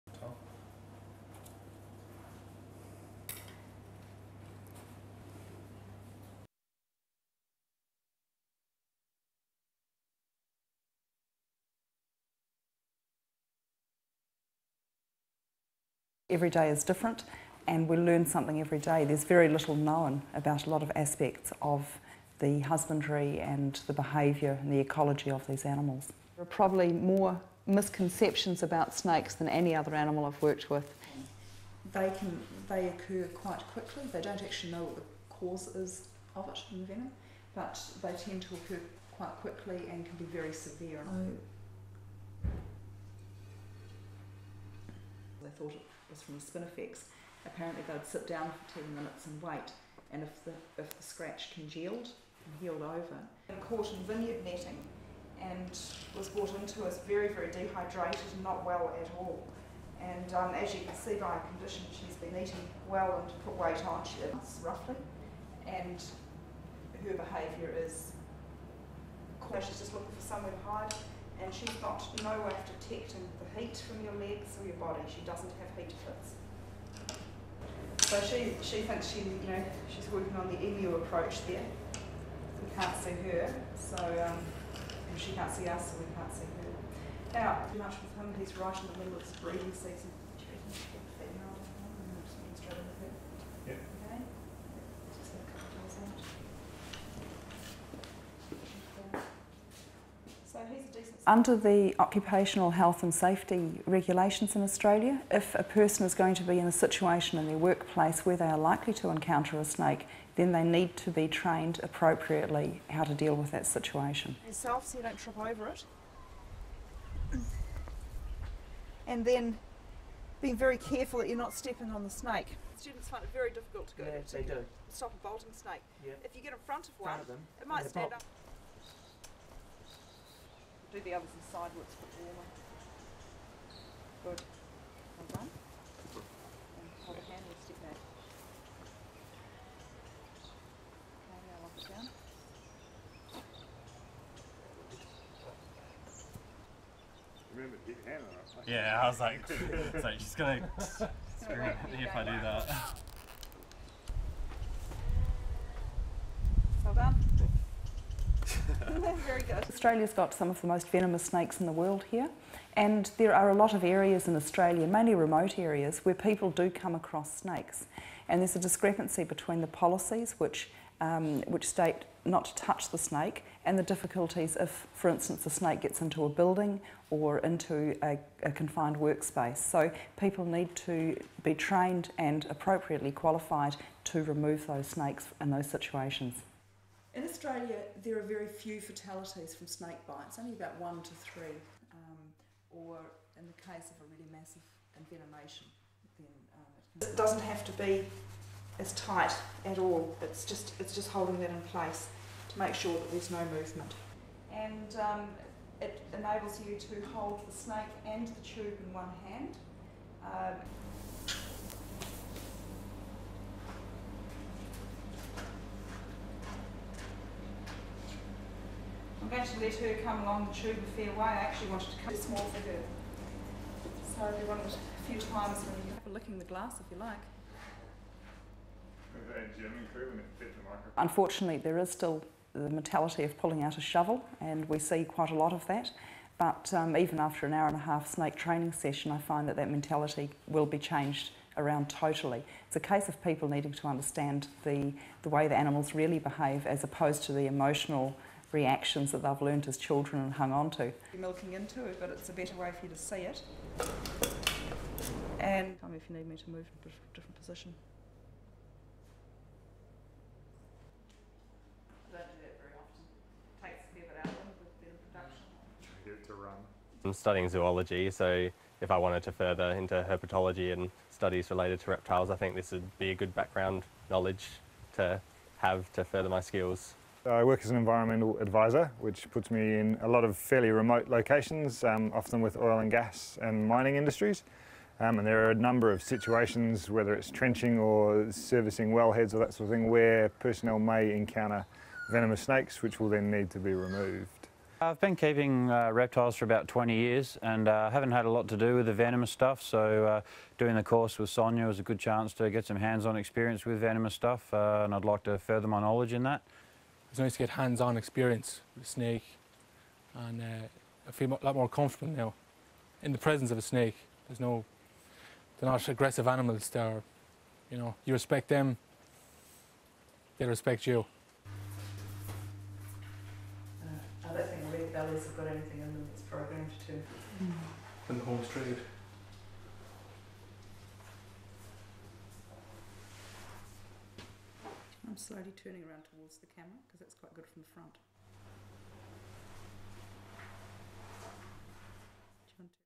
Demand for snake handling courses on rise in Australia
AP Television
Echunga, South Australia, 29 August 2009
1. Close of researcher and breeder Sonia Dewdney feeding mouse to snake using tongs, zoom into snake biting mouse
2. Various of Dewdney examining lizard eggs using torch
3. SOUNDBITE (English) Sonia Dewdney, reptile researcher and breeder:
"Every day is different, and we learn something every day. There's very little known about a lot of aspects of the husbandry and the behaviour and the ecology of these animals."
4. Wide of snake-handling course classroom session, UPSOUND (English): Sonia Dewdney: "There are probably more misconceptions about snakes than any other animal I've worked with."
5. Pan across course students sitting at table listening
6. Close of graph on computer screen listing snake bite fatalities linked to various species
7. Wide of students listening
8. Wide of snake on floor in front of students
9. Close of snake crawling towards student's boots
10. Various of snake crawling over and around students' boots
11. SOUNDBITE (English) Sonia Dewdney, reptile researcher and breeder:
"Under the occupational health and safety regulations in Australia, if a person is going to be in a situation in their workplace where they are likely to encounter a snake, then they need to be trained appropriately how to deal with that situation."
12. Wide of Dewdney demonstrating how to catch snake
13. Close of snake on grass
14. Various of student Matt Galebiowski learning how to catch snake
15. SOUNDBITE (English) Sonia Dewdney, reptile researcher and breeder:
"Australia's got some of the most venomous snakes in the world here, and there are a lot of areas in Australia, mainly remote areas, where people do come across snakes. And there's a discrepancy between the policies, which state not to touch the snake, and the difficulties if, for instance, the snake gets into a building, or into a confined workspace, so people need to be trained and appropriately qualified to remove those snakes in those situations."
16. Wide of Dewdney speaking during classroom session
17. Close of computer screen listing symptoms of snake bites
18. Wide of Dewdney demonstrating first aid response to snake bite
19. Wide of Dewdney showing students snake-catching tube
20. Various of Dewdney catching snake using tube
21. Wide of students looking at snake in tube
22. Close of snake's head sticking out of tube
23. SOUNDBITE (English) Sonia Dewdney, reptile researcher and breeder:
"Unfortunately there is still the mentality of pulling out a shovel and we see quite a lot of that, but even after an hour and a half snake training session I find that that mentality will be changed around totally. It's a case of people needing to understand the way the animals really behave, as opposed to the emotional reactions that they've learnt as children and hung onto."
24. Various of Dewdney holding snake next to plastic-covered glass to 'milk' venom
25. Close of venom in bottom of glass
26. SOUNDBITE (English): Matt Galebiowski, snake-handling course student:
"I'm studying zoology so if I wanted to further into herpetology and studies related to reptiles, I think this would be good background knowledge to have, to further my skills."
27. SOUNDBITE (English): Max Dewdney, snake-handling course student:
28. SOUNDBITE (English): Marcus Raynor, snake-handling course student:
29. SOUNDBITE (English): Kevin Dalton, snake-handling course student:
30. Close of snake eating mouse
31. Tilt down from Dewdney watching snake to close of snake eating mouse
LEAD IN:
STORYLINE:
Keyword wacky animals